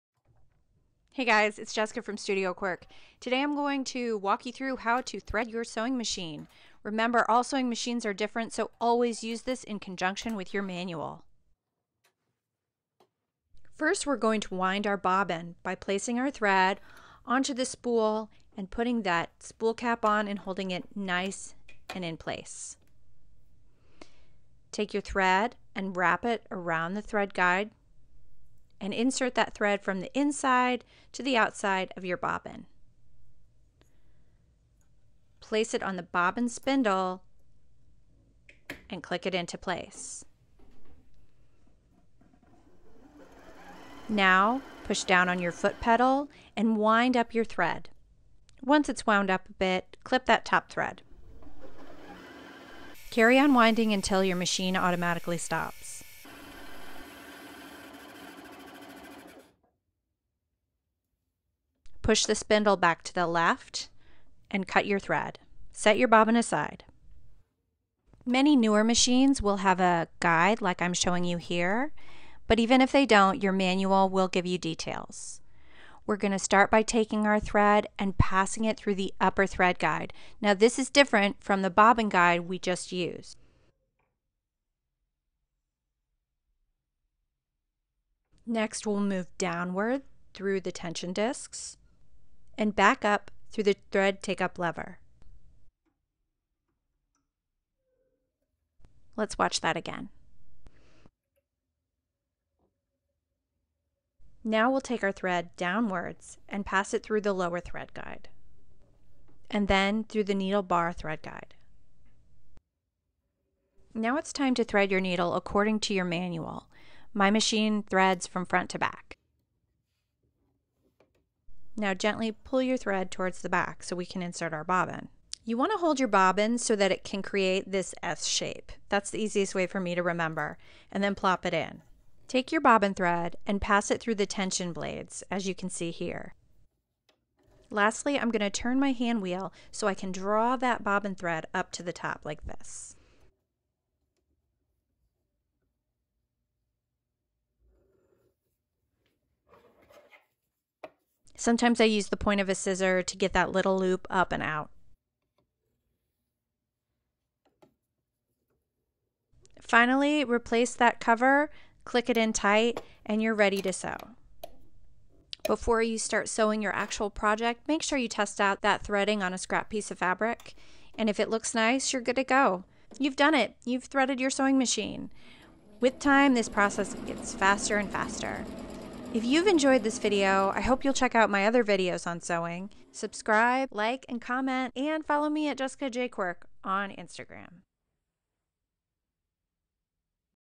How to Thread a Sewing Machine (and Wind a Bobbin)
Jessica demonstrates how to thread a modern home sewing machine. Use this video TOGETHER with your manual, as they may be variations between machines.

GLOSSARY OF TERMS used in this video:

Bobbin: Small spool of thread used to create the underside of your stitch
Spool Pin: Holds main spool of thread
Spool Holder: Used to keep thread on spool pin
Winder Thread Guide: Helps keep tension smooth when winding a bobbin
Bobbin Spindle: Holds bobbin case for winding
Tension Disc: Adjustable plate in your machine that can tighten or loosen tension of thread
Thread Take Up Lever: Holds thread between tension discs and lower thread guides.
Lower Thread Guide: Keeps thread in line to travel to needle
Needle Bar Thread Guide: Keeps thread in line to travel to needle

Important Tips!
- Failure to thread your machine according to the manufacturer's guidelines will result in messy, tangled threads or machine malfunction.
- Lift your presser foot before threading.
- Keep foot AWAY from pedal OR turn off machine while threading.
- Always test your threading on a scrap piece of fabric.
- Adjust your tension so stitch lines are even on top and bottom of seam.
- Change your needle with each project.

Have more sewing questions? Ask me in the comments below!